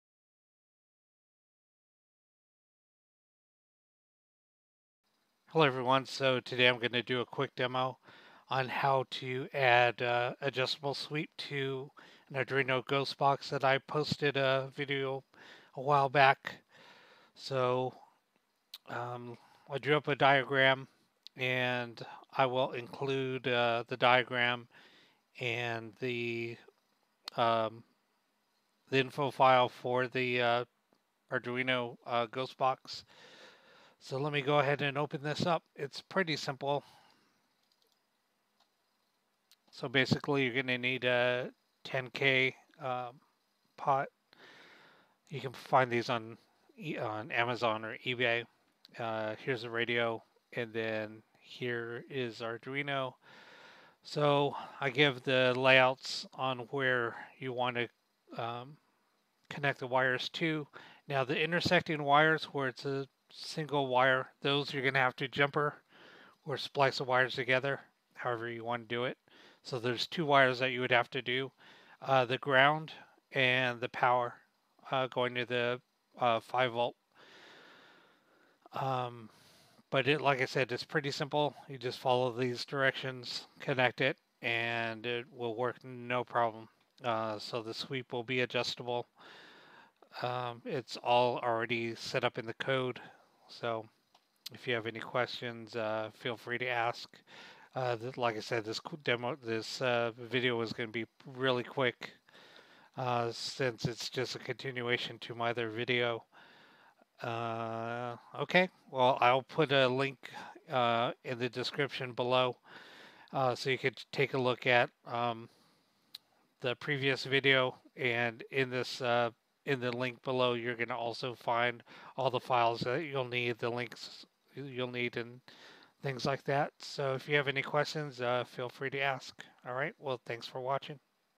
How to add Adjustable sweep to AGB
In this video I will walked you through adding adjustable sweep to an Arduino ghost box.
The code for the The Parawakening Spirit Box can be downloaded 100% free, no registration of any sorts is needed!
You are allowed to build or copy the device for private use only. Any commercial use or manufacturing is not allowed and prohibited! If you use codes or schematics for further videos i would be grateful if you would mention my channel.
Parts needed for the spirit box:
1 Arduino Uno
1 Jumper wires
1 TEA5767 FM radio
1 10k potentiometer
1 9V PP3 DIY Battery Clip With 2.1mm X 5.5mm DC Plug End
Hope you have fun building your own sweeping Ghost Box!